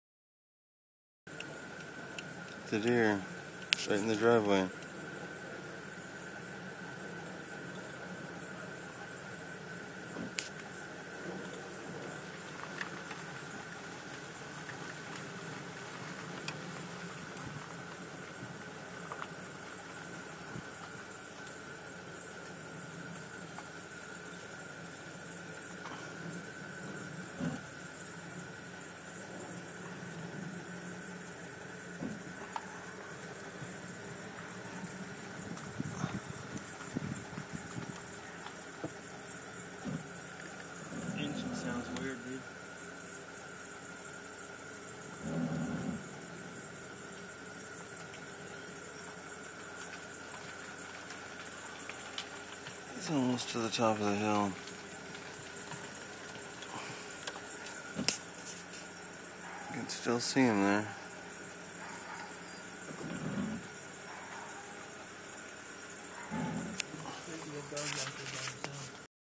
Deer on driveway
Going down the driveway we spotted this beautiful creature. Keep it real folks!